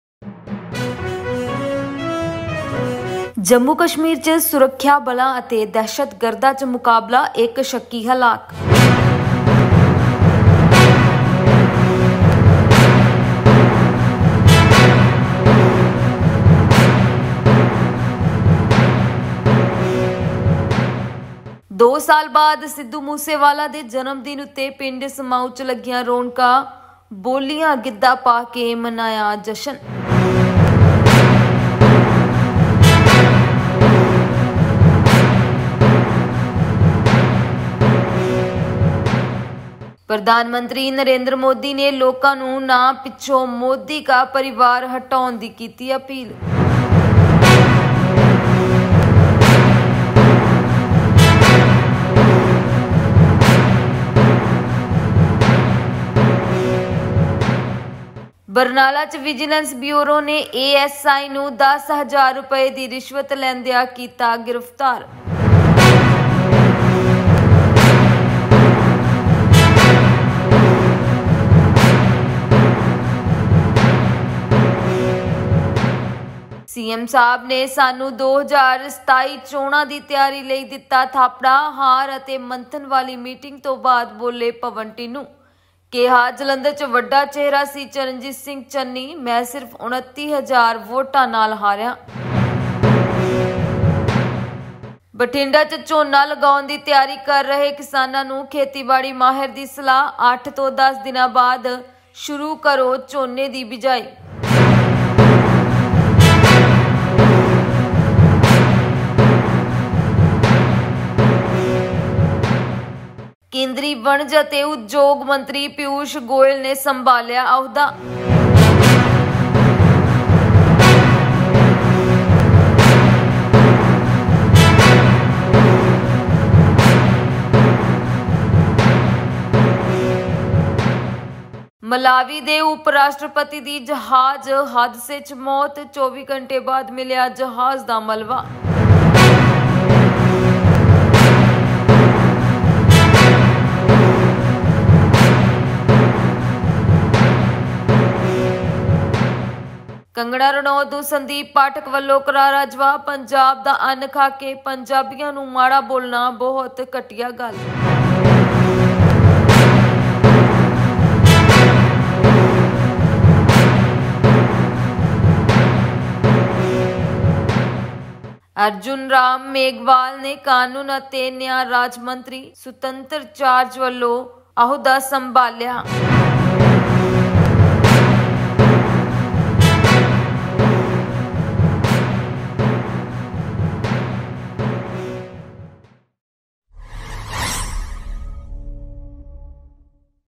'CM ਸਾਬ੍ਹ ਨੇ ਸਾਨੂੰ 2027 ਚੋਣਾਂ ਦੀ ਤਿਆਰੀ ਲਈ ਦਿੱਤਾ ਥਾਪੜਾ'! - NEWS UPDATE -TBN
Editor: Mandeep Maan